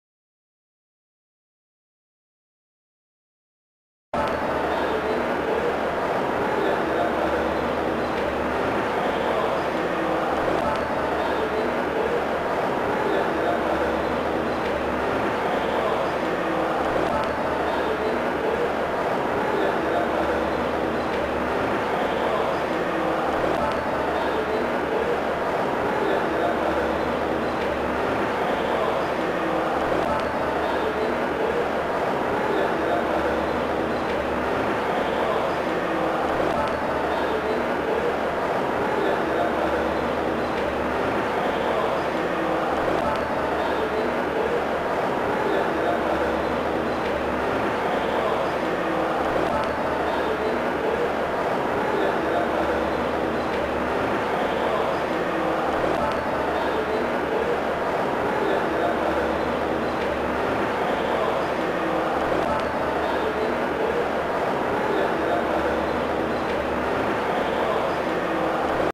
Charles Sandison
Visti da Artblog:  Charles Sandison (video-installazione, durata del video: 1 anno), galleria Yvon Lambert, ARTEFIERA (Bologna, 2007) -